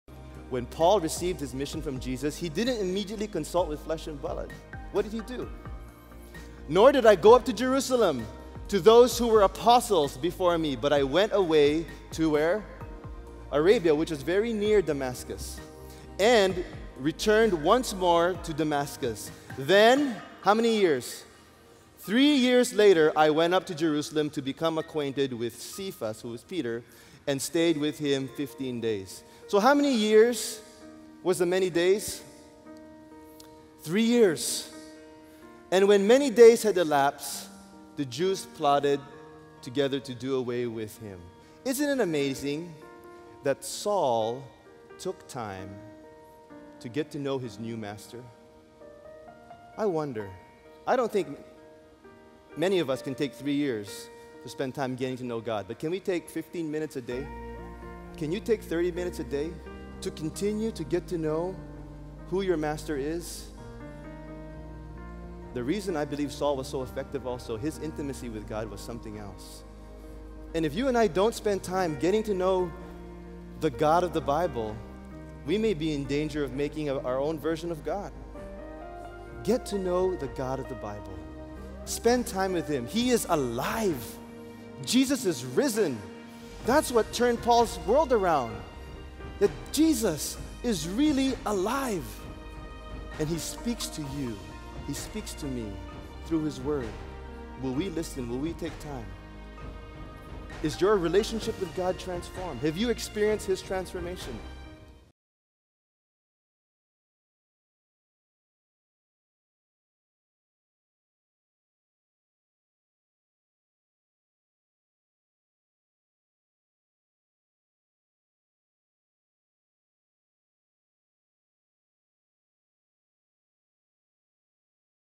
Is Your God The God of The Bible? - Paul Tanchi - Truth Matters Snippets
If we don't spend time getting to know the God of the Bible, we could be in great danger of creating our own version of God.

This video is a portion of last Sunday's message entitled, "Experience God's Transformation".